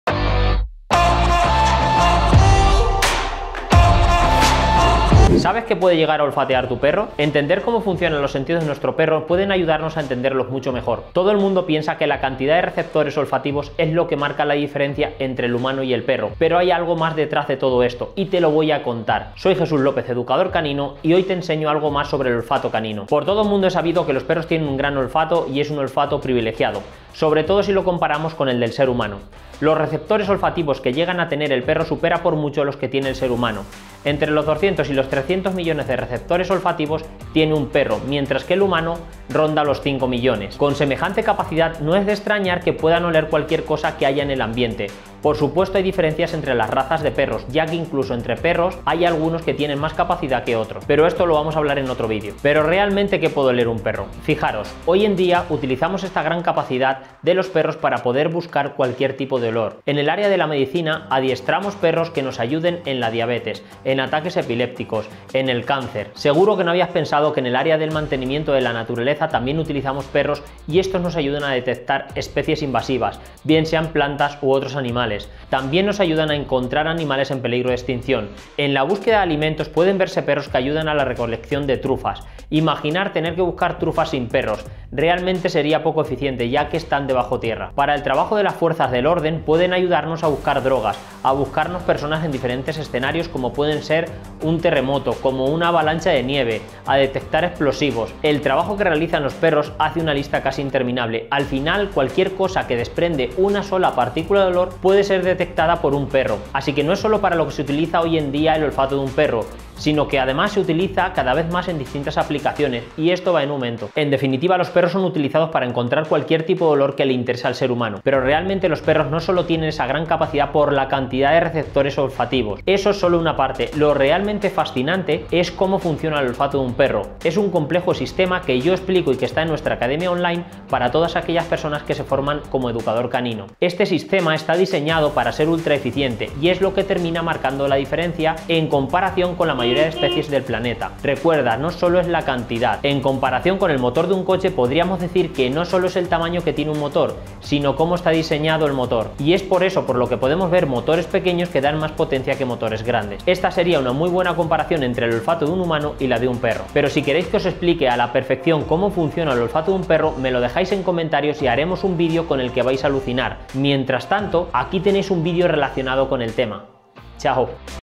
🐽OLFATO del PERRO ▶ ¿QUÉ OLFATEAN los PERROS? (Esto No lo Sabías)
Alguna vez te has preguntado cómo de efectivo es el OLFATO de los PERROS, QUÉ OLFATEAN LOS PERROS? Pues este vídeo va más allá. Te cuento desde la cantidad de receptores olfativos hasta las utilidades que tiene esta capacidad con la que ha nacido tu perro. ¿ Te interesa?, pues adelante🐽💥
0:00 - Inicio
0:29 - Olfato del perro
0:52 - ¿Qué puede oler un perro?
2:05 - ¿Como funciona el olfato del perro?